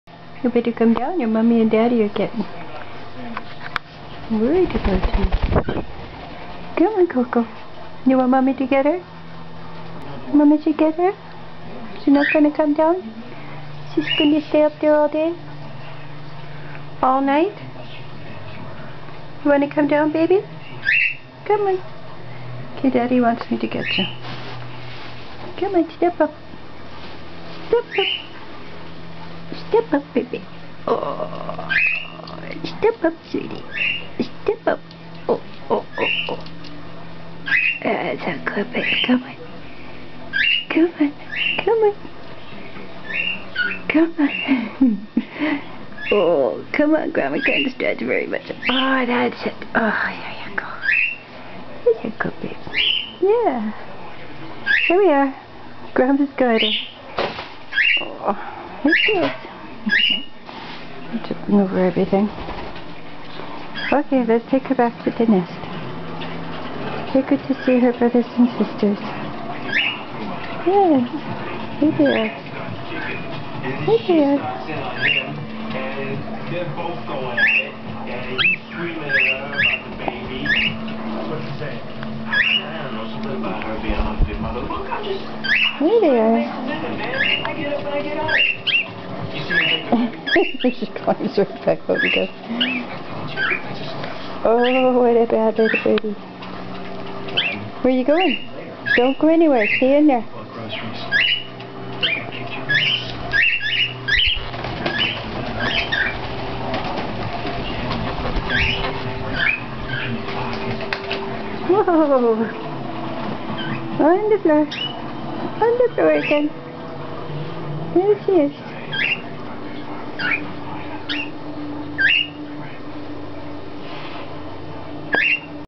Can't Get Off
1st baby can fly but gets scared and won't move.  Have to do a rescue.